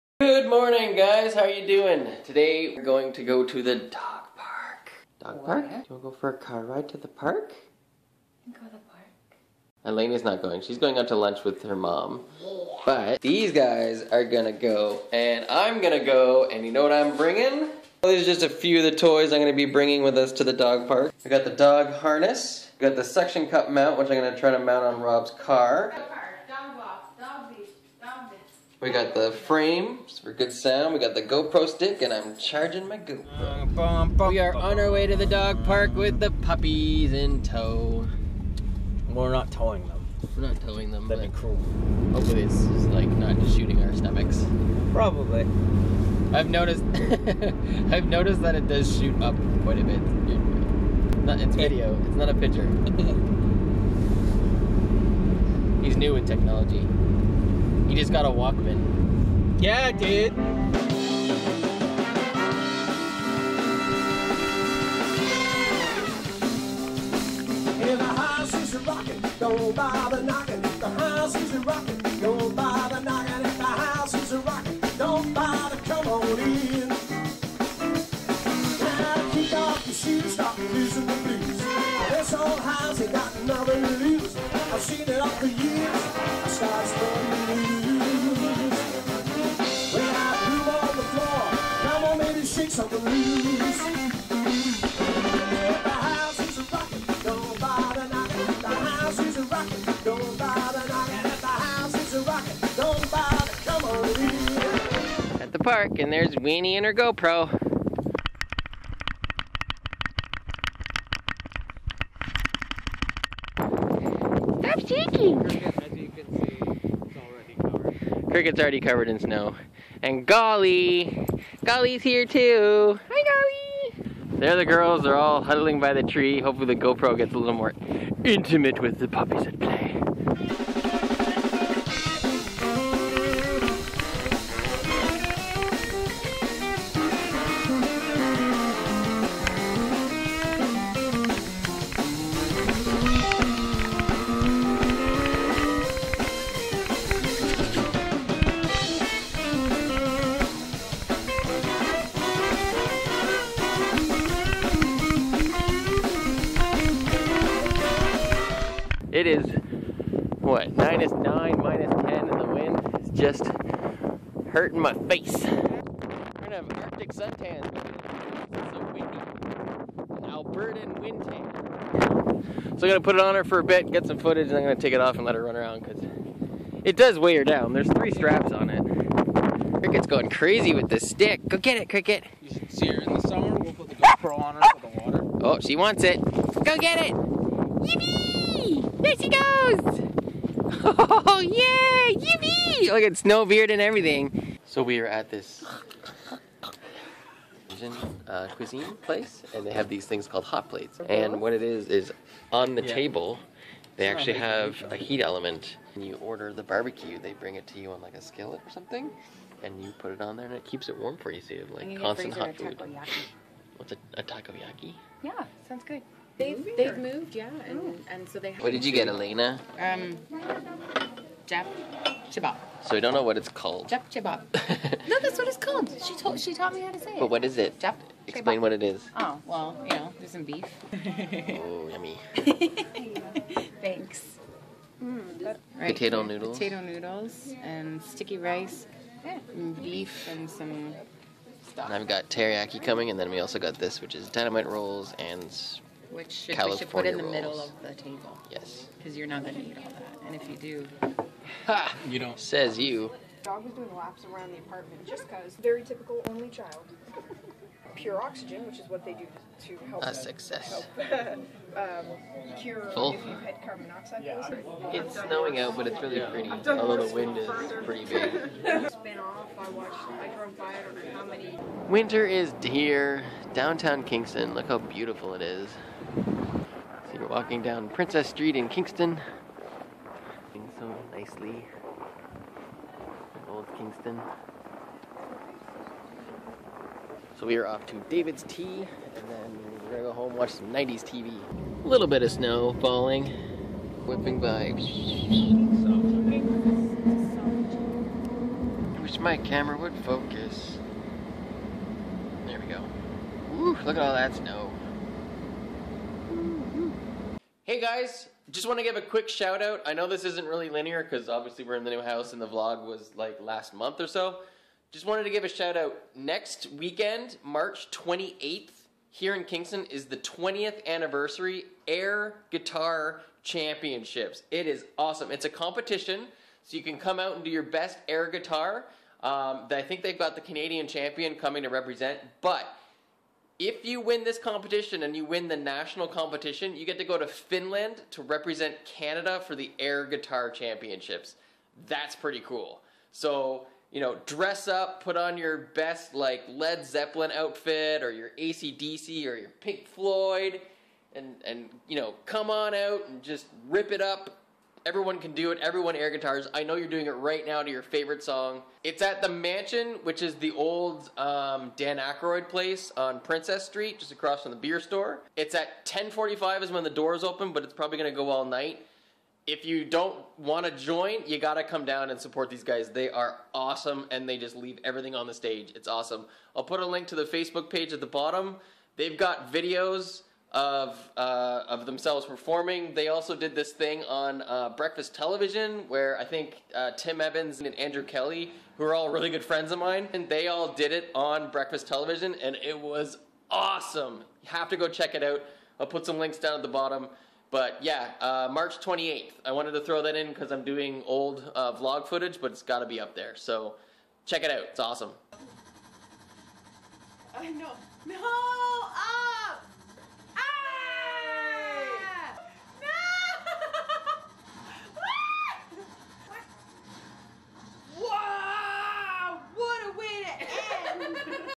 SNOWPro!! - #44
We take the dogs to the dog park, and I try out the GoPro in the Snow!!  Lots of great footage of the puppies from Winnie's POV. Finished the day at a great vietnamese place with HOT plates!

Canadian Air Guitar Championships!